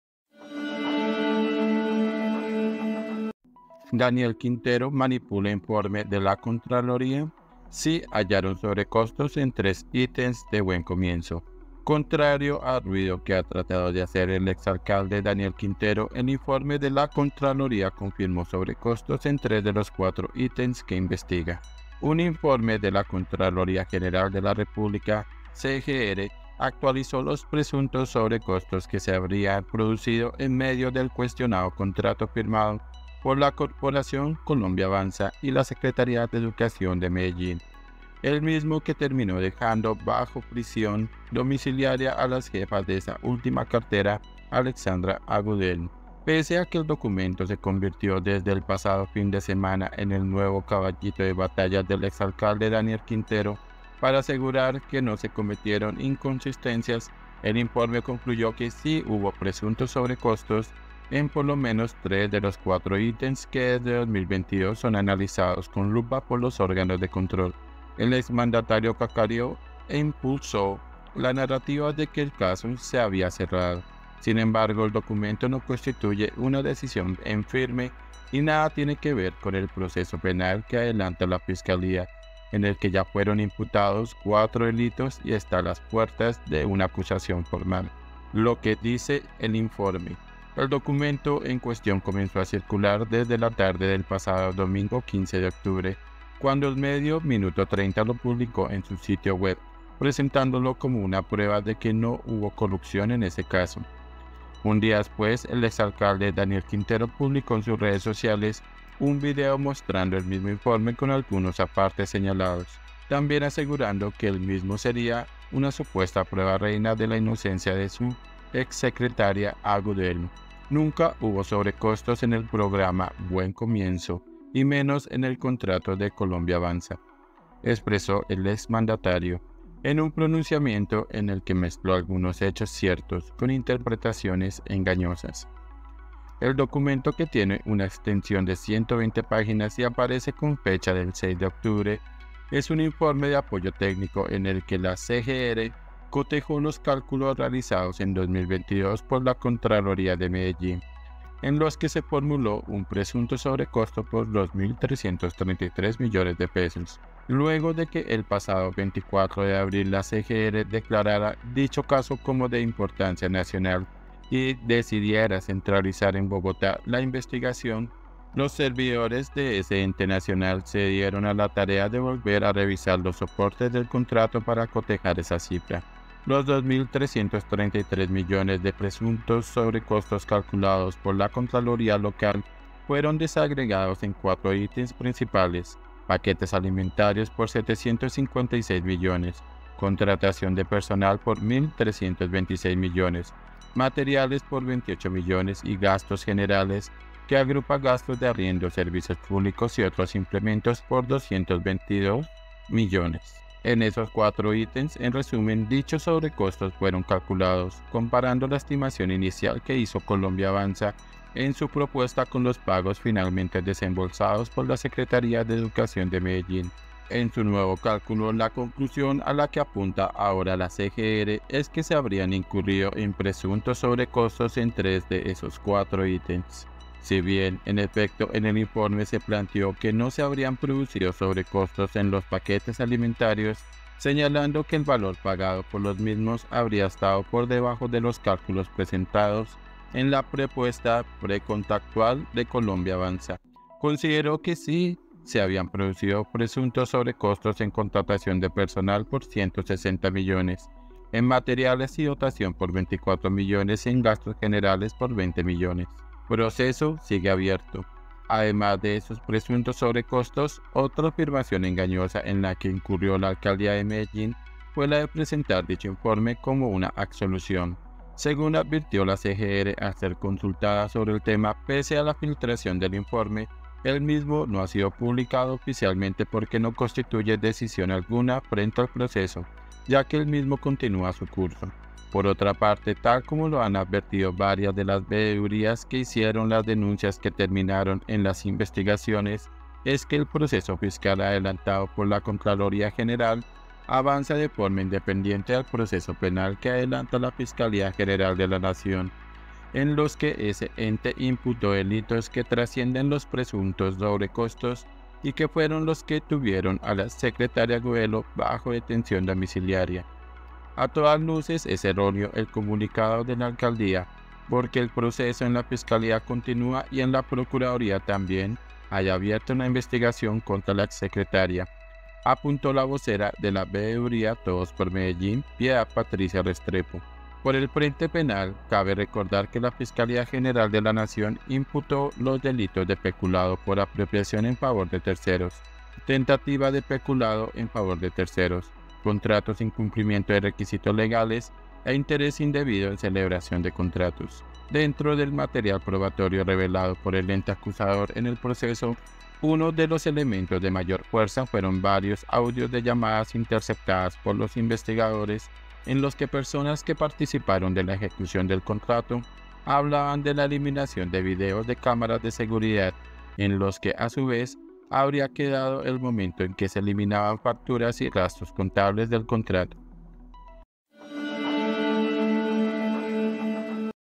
DENUNCIA 🛑 ¿DANIEL QUINTERO MANIPULA INFORMA DE LA CONTRALORÍA?
¿Daniel Quintero manipula informe de la Contraloría? Sí hallaron sobrecostos en tres ítems de Buen Comienzo
Contrario al ruido que ha tratado de hacer el exalcalde Daniel Quintero, el informe de la Contraloría confirmó sobrecostos en tres de los cuatro ítems que investiga.